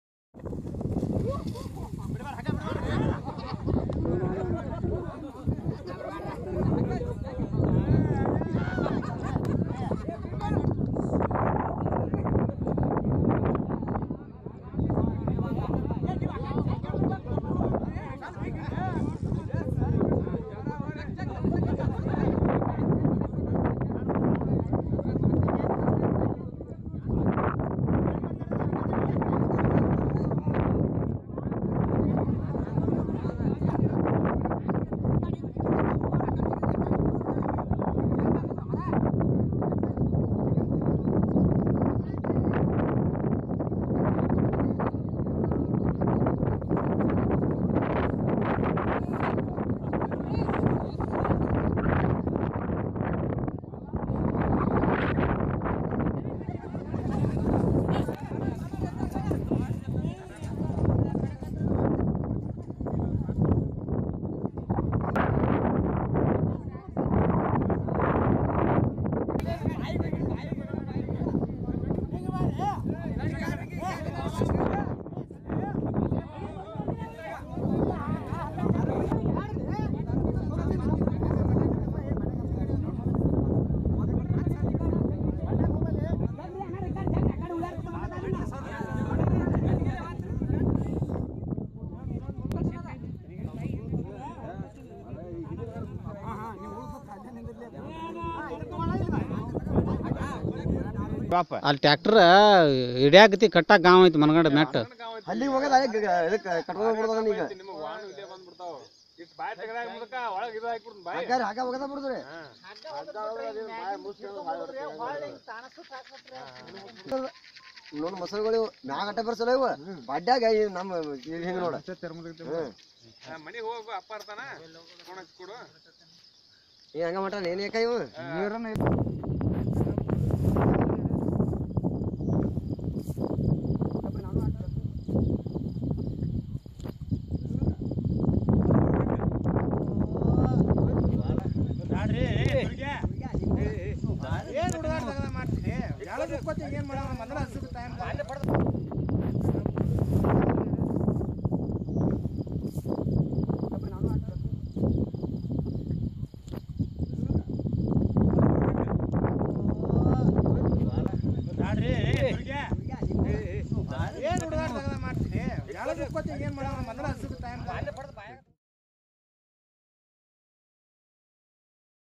Containing the beast - Crocodile in Bagalkot
In a Karnataka village, a group of men surrounded a crocodile, which was forced to come out of the river and go to the village in search for water and food during the fast-depleting reserves of the Krishna River. The attempt of containing the croc is not an easy task.

The fast-depleting reserves of the Krishna River, which flows through a large part of Bagalkot and Vijayapura districts, have not only resulted in a drinking water problem in the two districts, but the situation has also made life difficult for animals. Three crocodile deaths have been reported at Bilagi taluk of Bagalkot district during May, 2017.

Moreover, what has added to villagers' anxiety is that the reptiles are also increasingly venturing out of the river - nine crocodiles have been rescued at Bilagi taluk in the last two weeks in search of food and water.